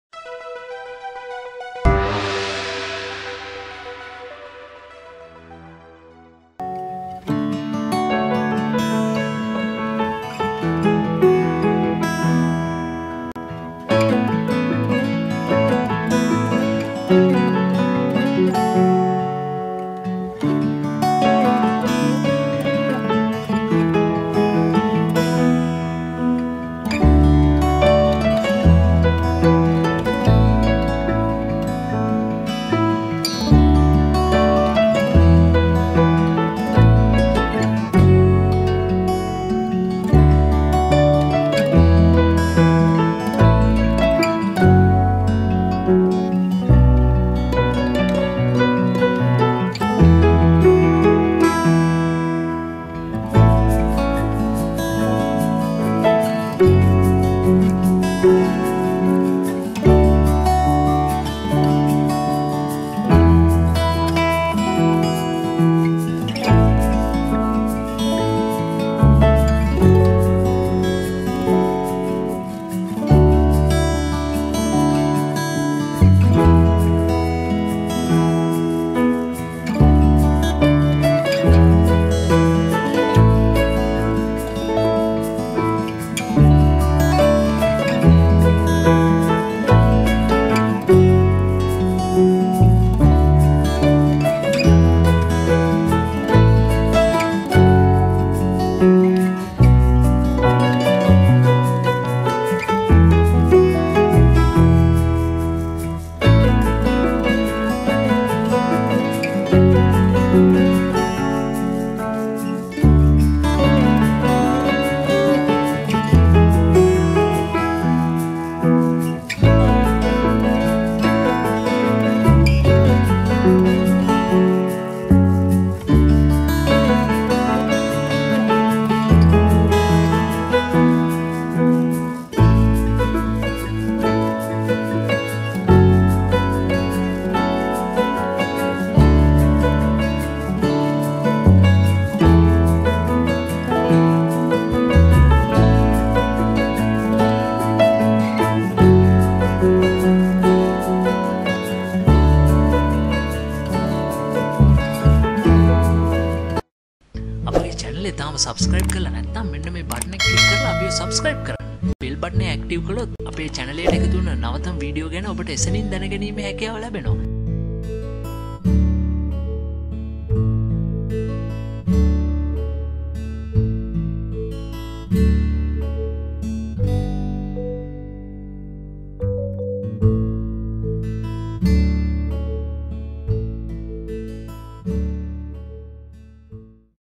Here are the latest pictures of Sajitha & Buvi's newborn son Jaden
අනේ .. මේ පුළුන් බෝලේ බලන්නකෝ..
සජිත බුවීගේ අලුත උපන් ජෙඩන් පුතු පැටියගේ අලුත්ම චායාරූප මෙන්න. Here are the latest pictures of Sajitha & Bhuvi's newborn son Jaden